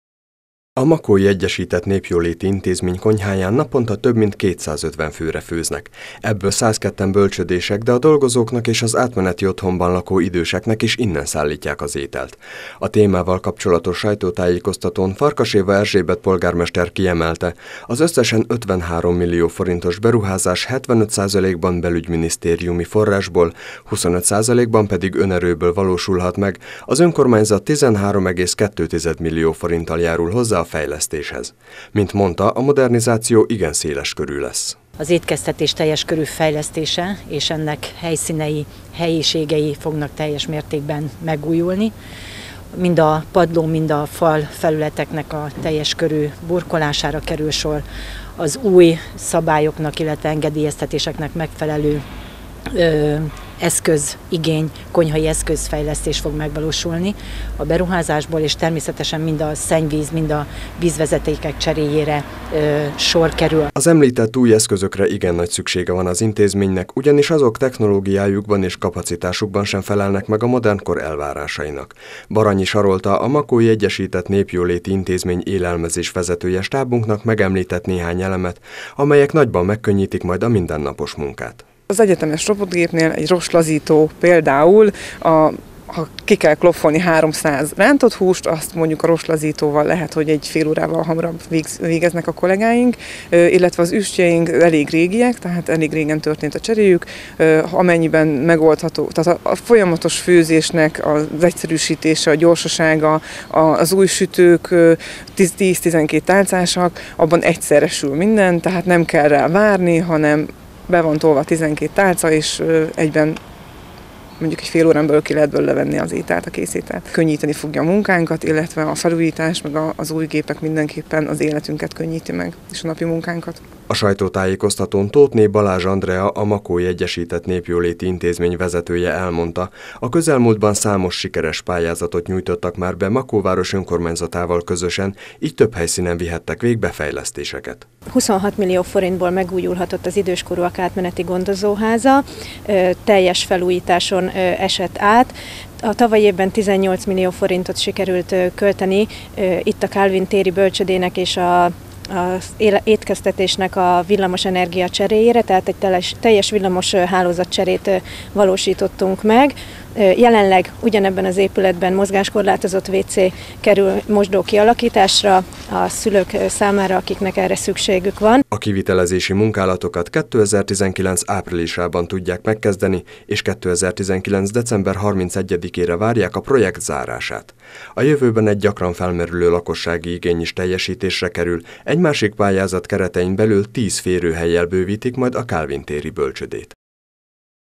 53 millió forintból újulhat meg a Makói Egyesített Népjóléti Intézmény konyhája
Többek között a vízvezetékrendszer teljes cseréjére és burkolási munkálatokra is sor kerül a közeljövőben a Makói Egyesített Népjóléti Intézmény konyhaépületében egy, az önkormányzattal közösen benyújtott belügyminisztériumi pályázatnak köszönhetően. A beruházás végén nem csak új környezetben folyhat majd a munka, de nagyobb kapacitású, modernebb gépek is könnyebbé teszik a mindennapokat.